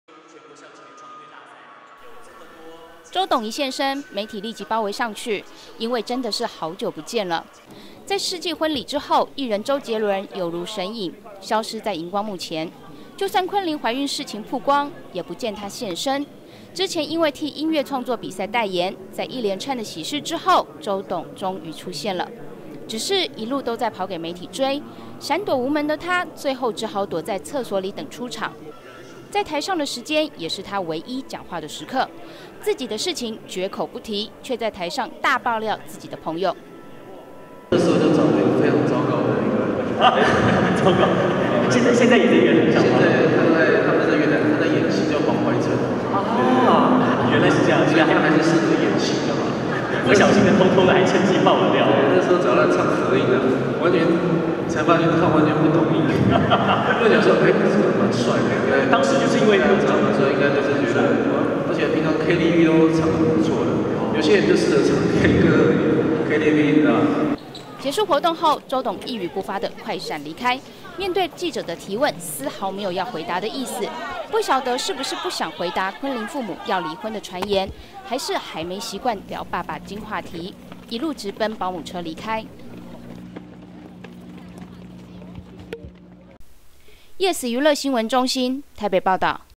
【5年前】不敢面對懷孕 周董逃竄演出追逐大戰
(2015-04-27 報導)

周董一現身，媒體立即包圍上去，因為真的是好久不見了，在世紀婚禮之後，藝人周杰倫有如神隱，消失在螢光幕前，就算昆淩懷孕事情曝光，也不見他現身，之前因為替音樂創作比賽代言，在一連串的喜事之後，周董終於出現了，只是，一路都跑給媒體追，閃躲無門的他，最後只好躲在廁所裡等出場，在臺上的時間也是他唯一講話的時刻。自己的事情絕口不提，卻在臺上大爆料自己的朋友。
結束活動後，周董一語不發的快閃離開，面對記者的提問絲豪沒有要回答的意思，不曉得是不是不想回答昆淩父母要離婚的傳言還是還沒習慣聊爸爸經話題，一路直奔保姆車離開